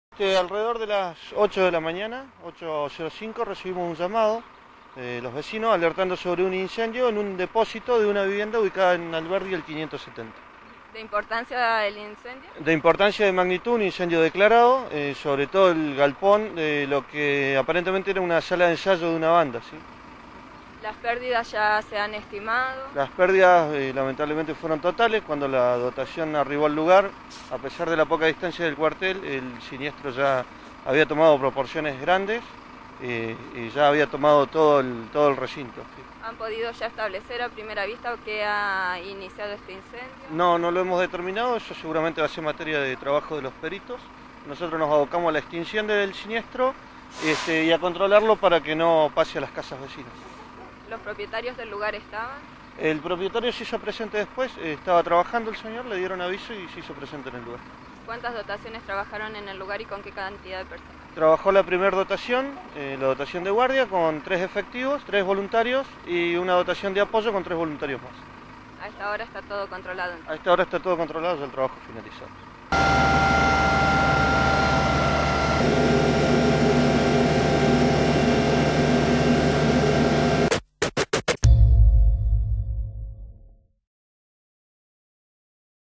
Walter Tapia Bomberos voluntarios
Incendio en galpón. Pérdidas materiales totales